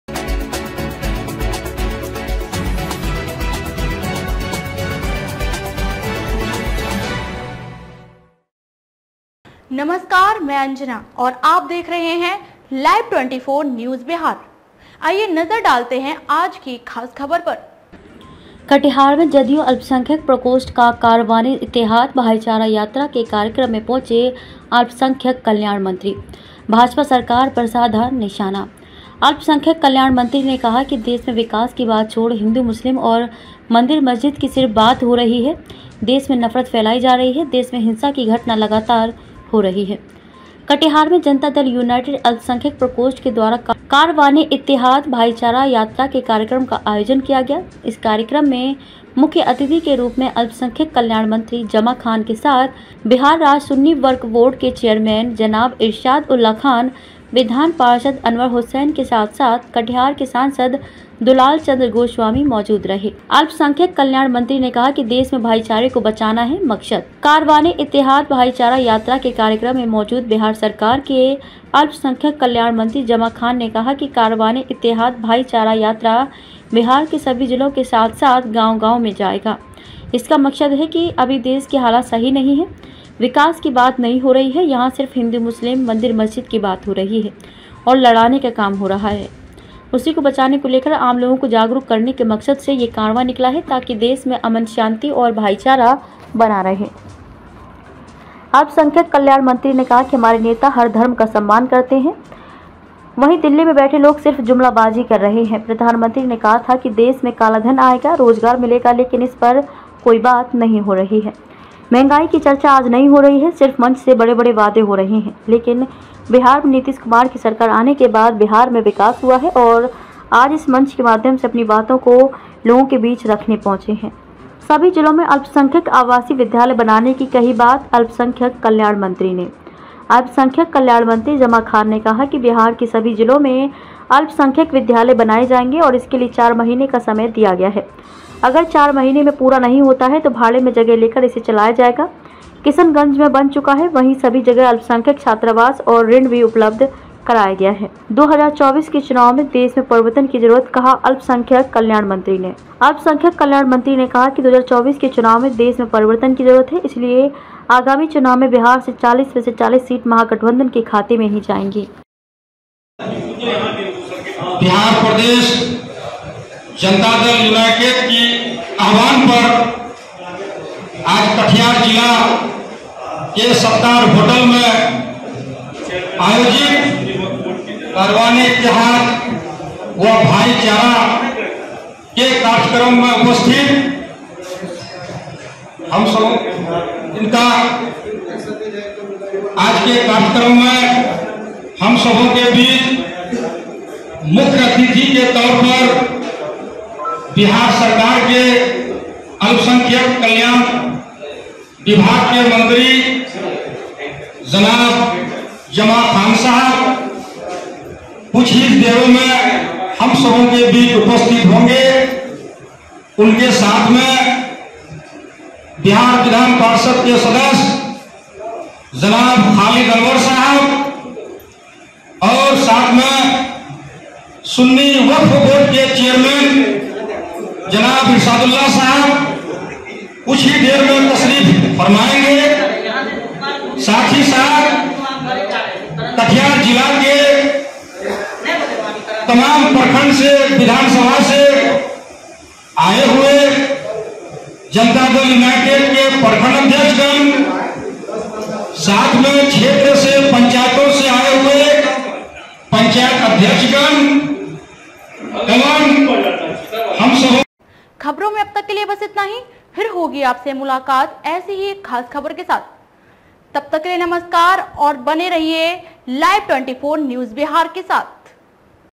कटिहार में जदयू अल्पसंख्यक प्रकोष्ठ का कारवांने इत्तेहाद- भाईचारा यात्रा के कार्यक्रम में पहुंचे?
कटिहार में जदयू अल्पसंख्यक प्रकोष्ठ का कारवांने इत्तेहाद- भाईचारा यात्रा के कार्यक्रम में पहुंचे अल्पसंख्यक कल्याण मंत्री।__भाजपा सरकार पर साधा निशाना
अल्पसंख्यक कल्याण मंत्री ने कहा देश में विकास की बात छोड़ हिंदू मुस्लिम और मंदिर मस्जिद की  सिर्फ बात बात हो रही है_देश में नफरत फैलाई जा रही है।देश में हिंसा की घटना लगातार हो रही है।_____रिपोर्ट तौकीर रजा कटिहार।

कटिहार में जनता दल यूनाइटेड अल्पसंख्यक प्रकोष्ठ के द्वारा कारवाने इत्तेहाद- भाईचारा यात्रा के कार्यक्रम का आयोजन किया गया। इस कार्यक्रम में मुख्य अतिथि के रूप में अल्पसंख्यक कल्याण मंत्री जमा खान के साथ बिहार राज्य सुननी वर्क बोर्ड के चेयरमैन जनाब इरशाद उल्लाह खान विधान पार्षद अनवर हुसैन के साथ-साथ
 कटिहार के सांसद दुलाल चंद्र गोस्वामी मौजूद रहे ।

(1)-अल्पसंख्यक कल्याण मंत्री ने कहा देश मे भाई चारे को बचाना है मकशद।
कारवाने इत्तेहाद भाई चारा यात्रा के कार्यक्रम में मौजूद बिहार सरकार के अल्पसंख्यक कल्याण मंत्री जमा खान ने कहा कि कारवाने इत्तेहाद-भाई चारा यात्रा बिहार के सभी जिलों के साथ साथ गाँव गाँव मे जाएगा,इसका मकशद है कि अभी देश के हालात सही नही है,विकास की बात नही हो रही है,यहां सिर्फ हिन्दू मुस्लिम, मंदिर मस्जिद की बात हो रही है और लड़ाने का काम हो रहा है।उसी को बचाने को लेकर आम लोगो को जागरूक करने के मकशद से ये कारवां निकला है।ताकि देश में अमन शांति और भाईचारा बनी रहे।
अल्पसंख्यक कल्याण मंत्री ने कहा कि हमारे नेता हर धर्म का सम्मान करते हैं।वहीं दिल्ली में बैठे लोग सिर्फ जुमलाबाजी कर रहे हैं,प्रधानमंत्री ने कहा था देश मे कालाधन आएगा,रोजगार मिलेगा लेकिन इसपर कोई बात नही हो रही है,मंहगाई की चर्चा आज नही हो  रही है सिर्फ मंच से बड़े बड़े वादे हो रहे हैं।लेकिन बिहार में नीतीश कुमार की सरकार आने के   बाद बिहार में विकास हुआ है और आज इस मंच के माध्यम से अपनी बातों को लोगो के बीच रखने पहुंचे हैं।

(2)-- सभी जिलों में अल्पसंख्यक आवासीय विद्यालय बनाने की कही बात कही अल्पसंख्यक  कल्याण मंत्री ने।
अल्पसंख्यक कल्याण मंत्री जमा खान ने कहा कि बिहार के सभी जिलों में अल्पसंख्यक विद्यालय बनाये जाएंगे और इसके लिए 4 महीने का समय दिया गया है,अगर 4 महीने में पूरा नही होता है तो भाड़े में जगह लेकर इसे चलाई जाएगी,किशनगंज में बन चुका है,वही सभी जगह अल्पसंख्यक छात्रावास और ऋण भी उपलब्ध कराई गई है।

(3)-2024 के चुनाव में देश मे परिवर्तन की जरूरत कहा अल्पसनाख्यक कल्याण मंत्री ने
अल्पसंख्यक कल्याण मंत्री ने कहा कि 2024 के चुनाव में देश मे परिवर्तन की जरूरत है,इसलिए आगामी चुनाव में बिहार से 40 में से 40 सीट महागठबंधन के खाते में ही जाएगी।bihar